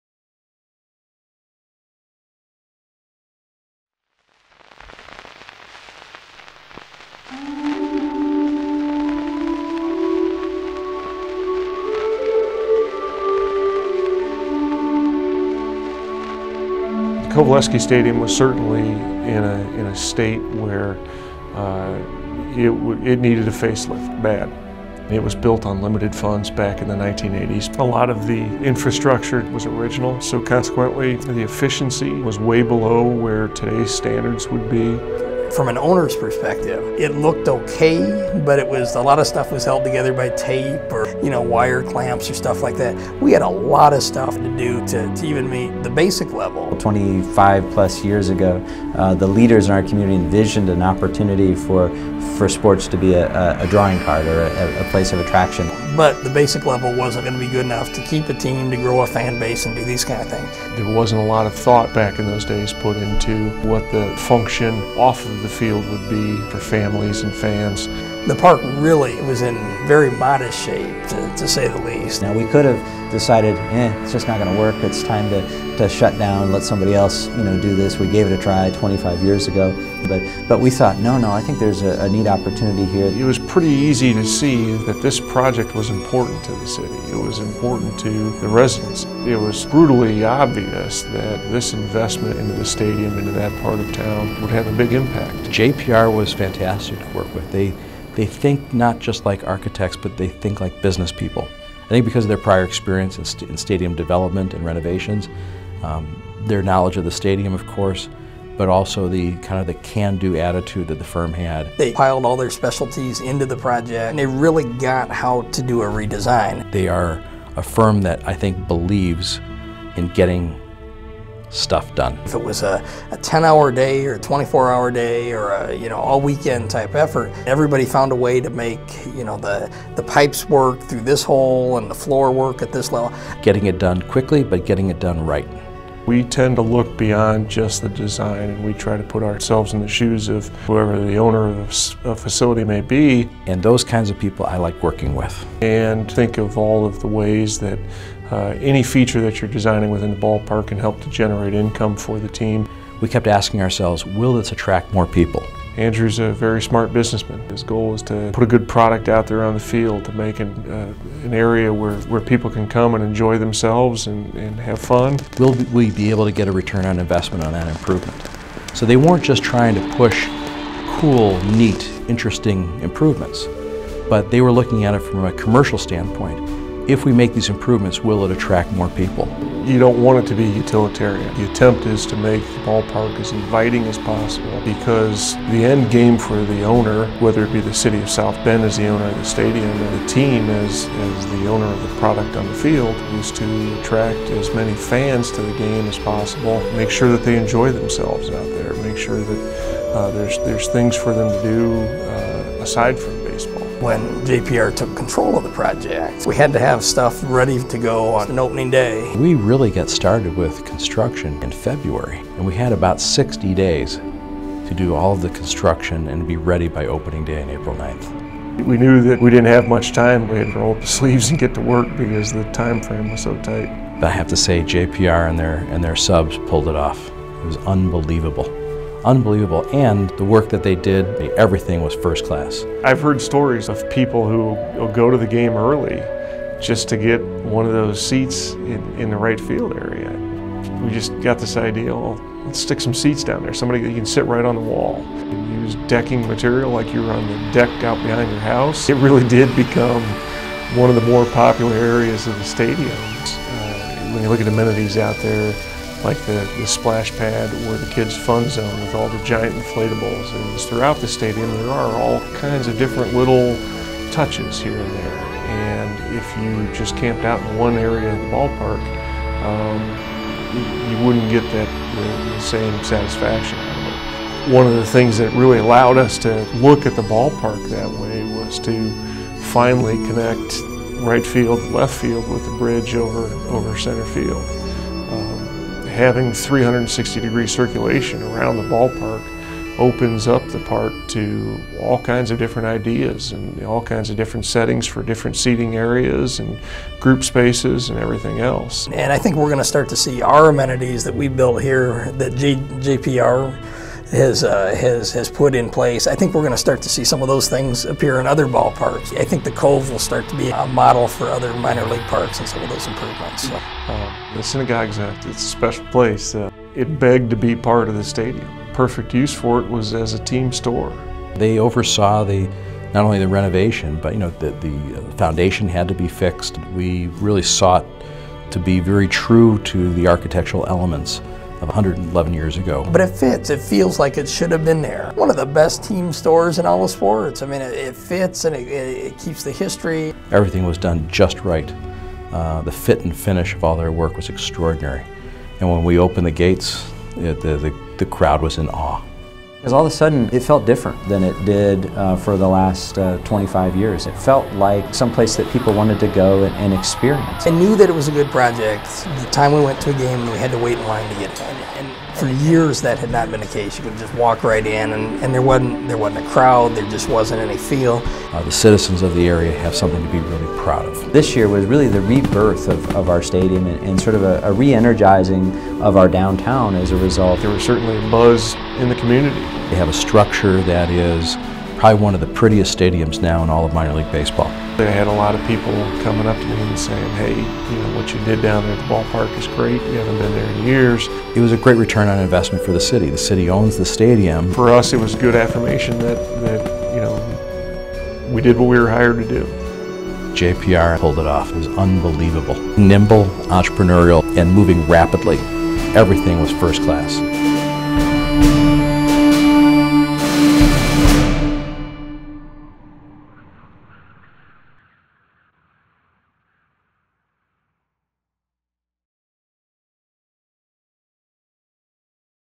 REQUIEM FOR A PARK
The rebirth of Stanley Coveleski Regional Baseball Stadium in South Bend, Indiana -- home to the South Bend Silver Hawks Minor League professional baseball team.  This film is presented by JPR, the architectural and design firm that specializes in this kind of stadium renovation.